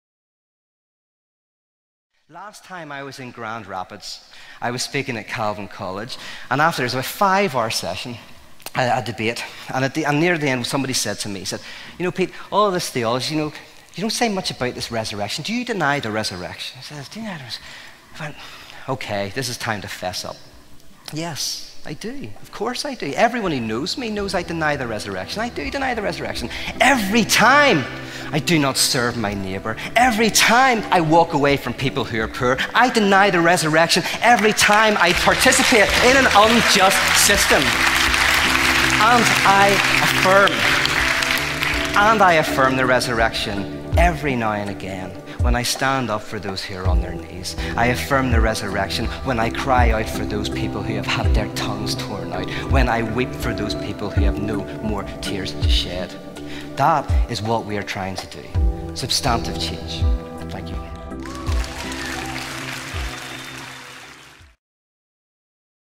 Peter Rollins - I Deny the Resurrection - NL (dutch) subs
Peter Rollins (Noord-Ierland) over hoe hij regelmatig de opstanding van de Christus ontkent.

Subs by Wouter Nieuwenhuizen
Blessings for this endeavour given by Peter Rollins himself via Twitter